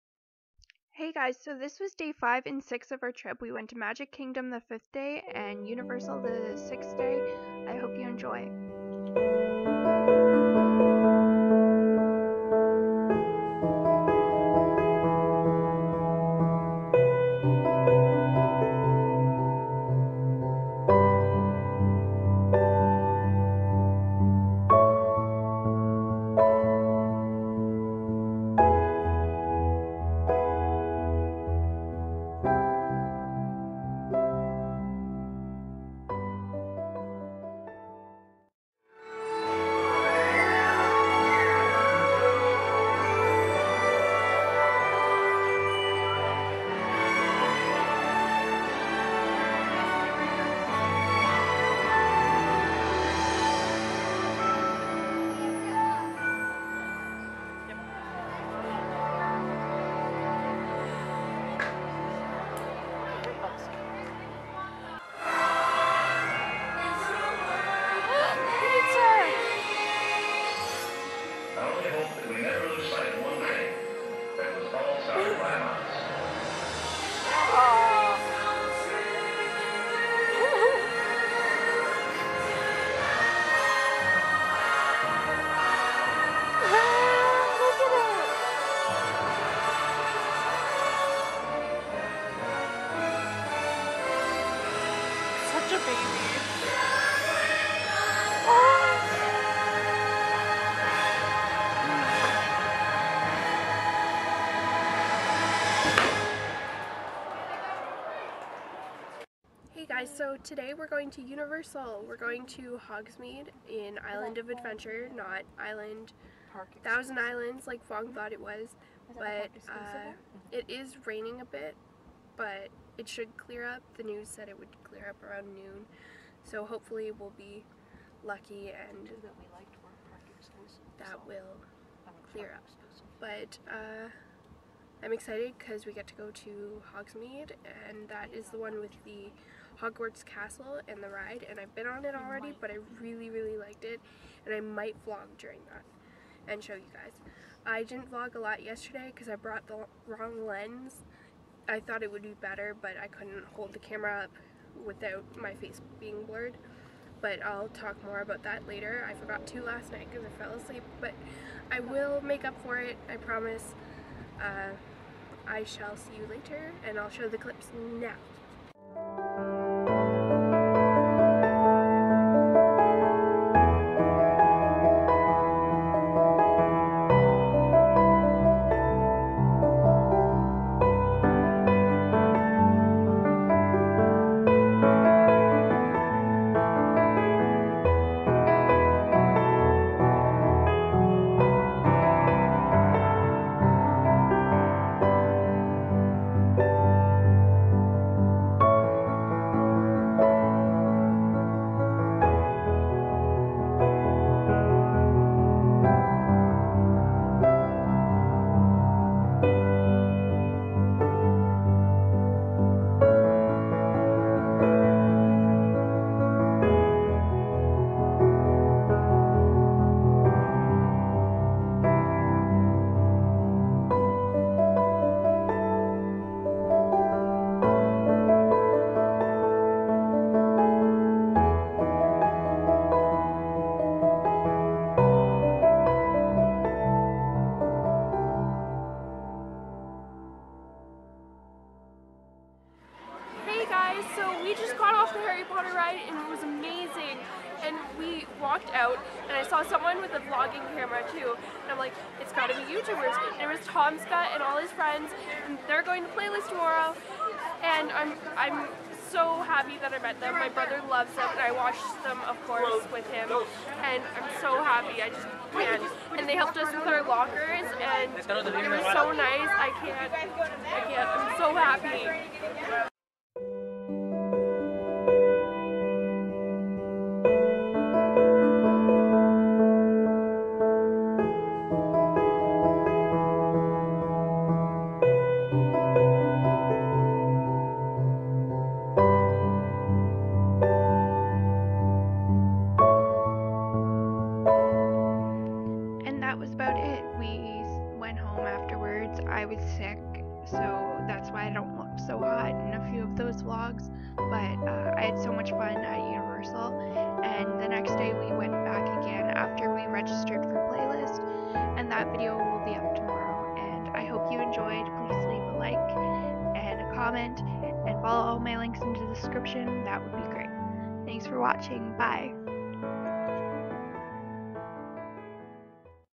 The Magic of Disney & Harry Potter | VLOG
The fifth and sixth day of our trip to Florida.